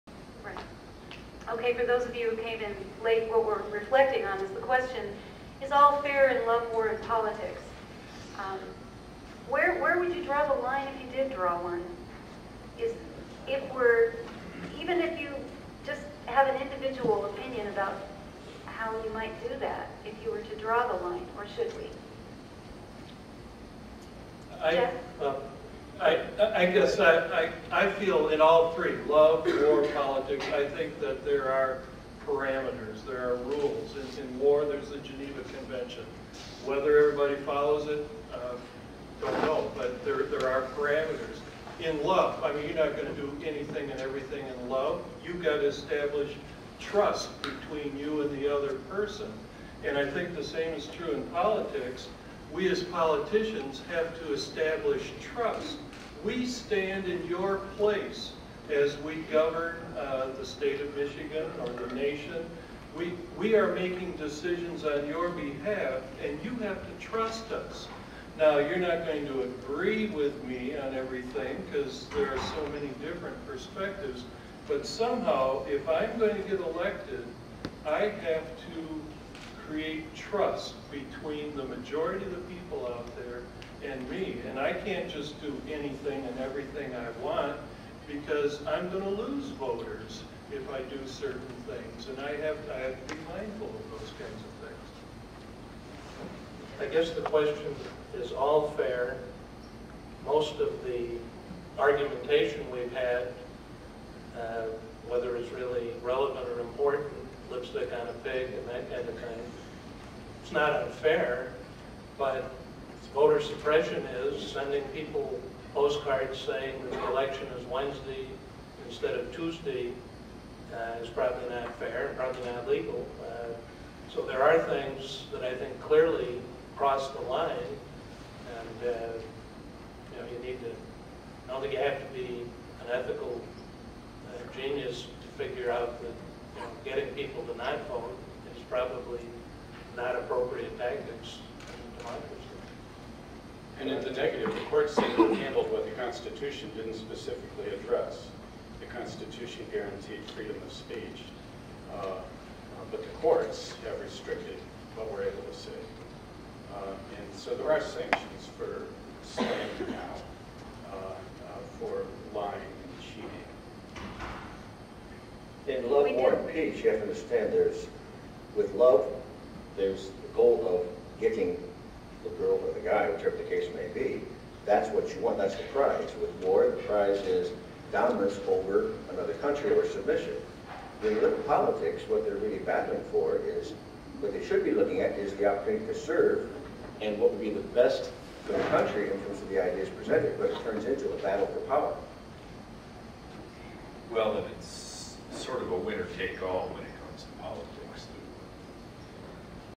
Civil Discourse
A panel on civil discourse at West Shore Community College.